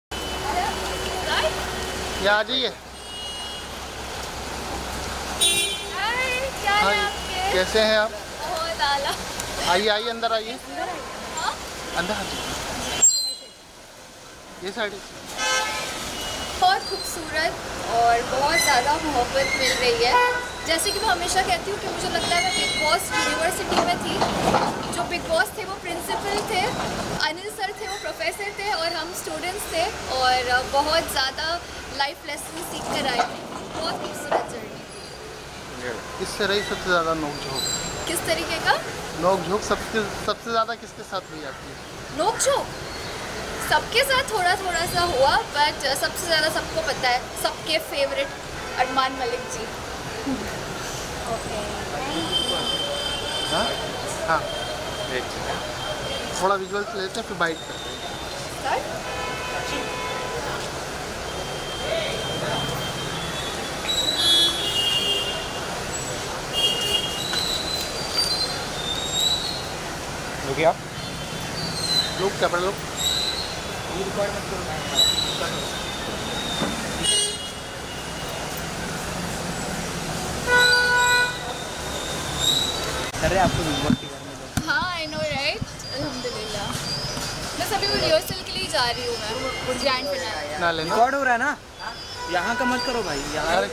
Bigg Boss OTT 3 star Sana Sultan spotted at dance rehearsals| Filmy Focus Bollywood
Bigg Boss OTT 3 star Sana Sultan spotted at dance rehearsals